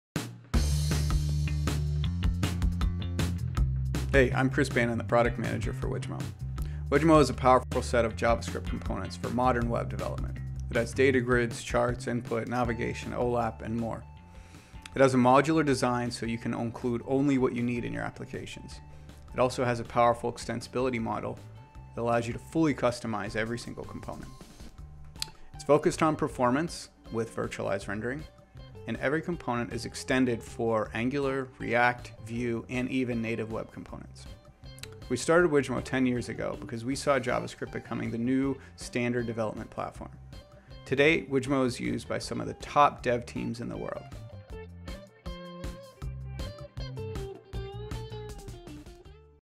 Wijmo - JavaScript UI Components for Enterprise Applications
Learn more about Wijmo from Product Manager, Chris Bannon. Wijmo is a suite of advanced JavaScript UI components designed for the modern web. With support for Angular, React, Vue, and Web Components, Wijmo is the leading suite of components for JavaScript developers.